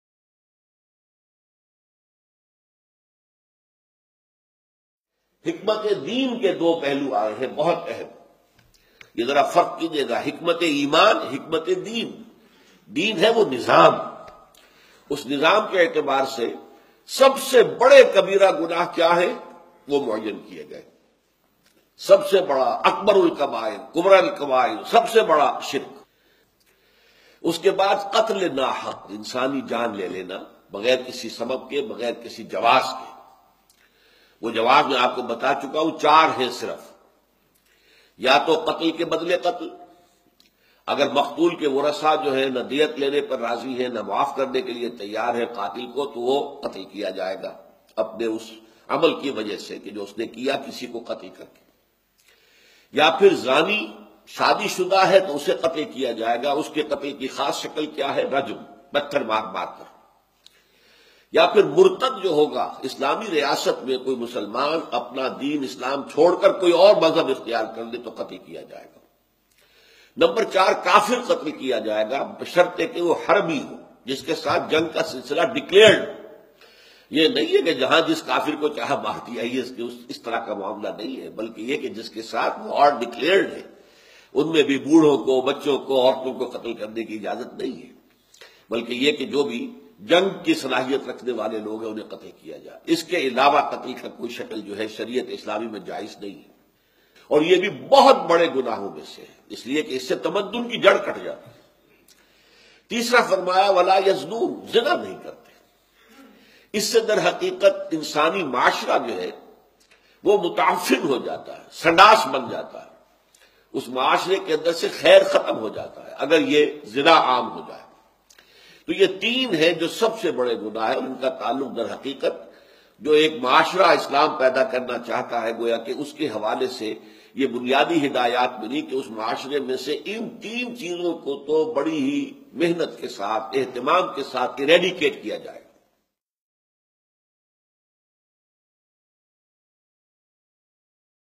اسلامی ریاست بڑے گناہوں کی سزا کے حوالے سے کیا معاملہ کرتی ہے ؟
How an Islamic State deals with major sins ?